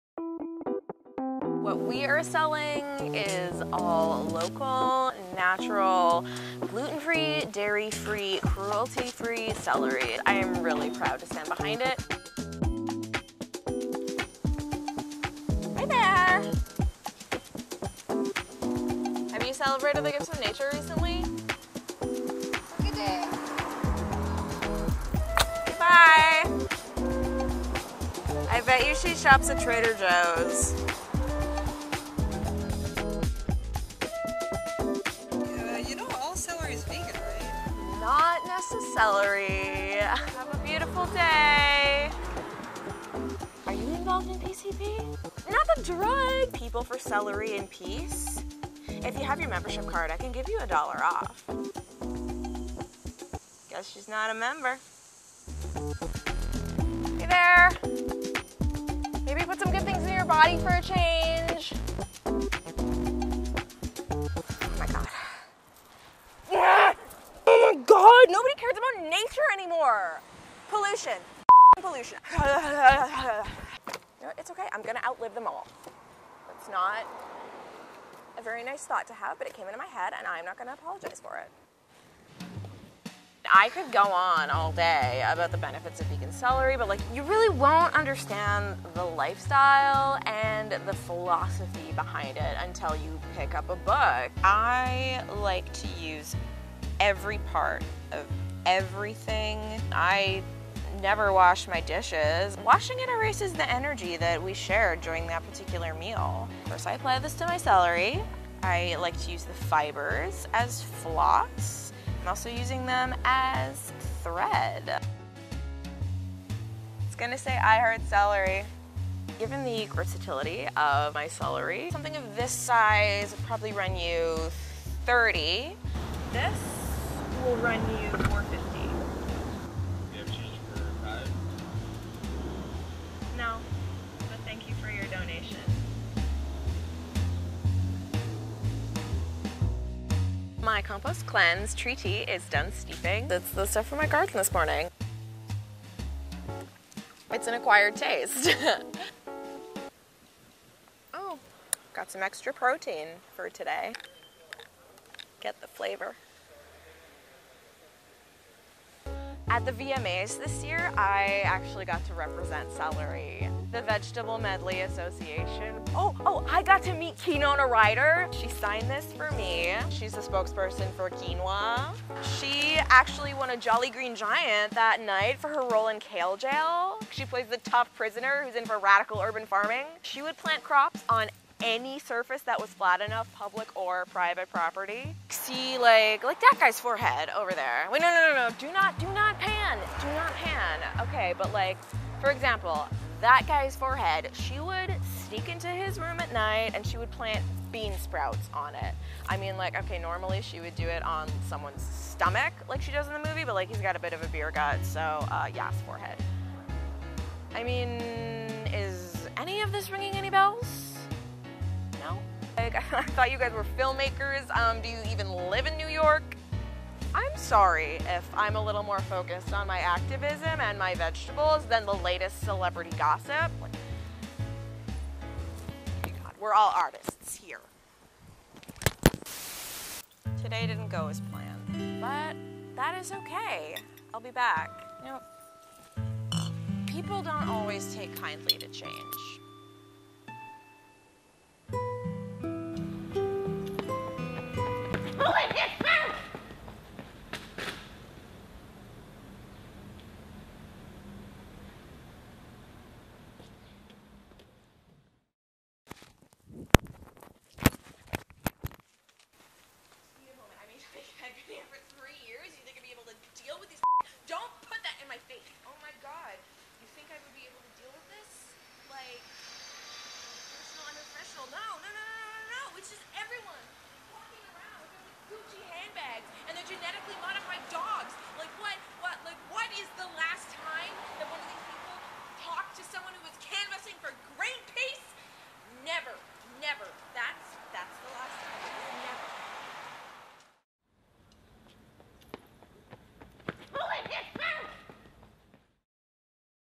Vegan Celery Episode 3 - Celerbration
Episode 3: Will Savannah celer-brate a celery success?

The table is set and the jars are filled! The series concludes as Savannah spends her first day as a legitimate Celswoman. But will she succeed in spreading the Vegan Celery lifestyle to the masses? Is the world ready?

Producer, Director, Writer: Sara Whittington / sarawhittington.com / Twitter @swhitt17 / Instagram @swhitt17 /

Videographer, Editor: Alice Obar / aliceobar.com / / Instagram @aliceobar